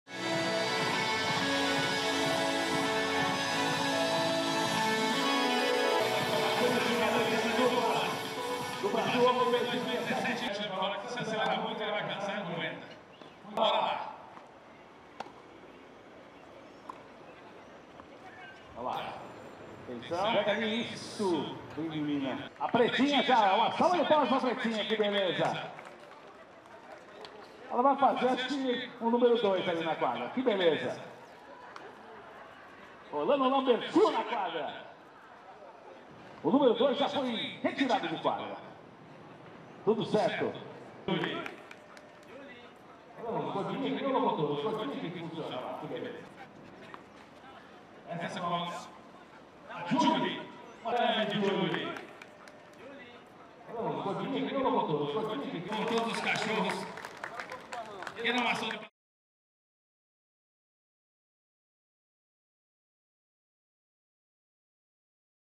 Brazil Open uses dogs as ball boys!
Tennis doubles specialist Marcelo Demoliner and Brazil Davis Cup captain Joao Zwestch shared the tennis court with some four-legged friends on Saturday (4th March) at the Brazil Open in Sao Paulo - as a group of rescue dogs were put into service as 'ball dogs'.